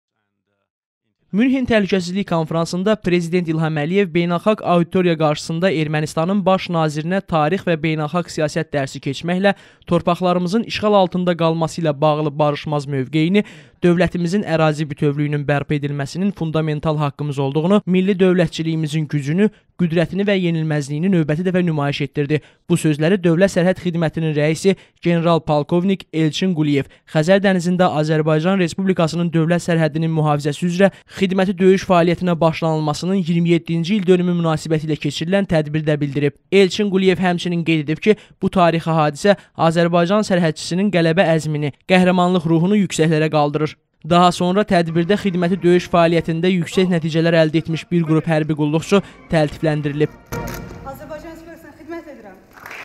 DSX-da tədbir keçirilib
Xəzər dənizində Dövlət Sərhədinin Mühafizəsinin 27-ci ili qeyd olunub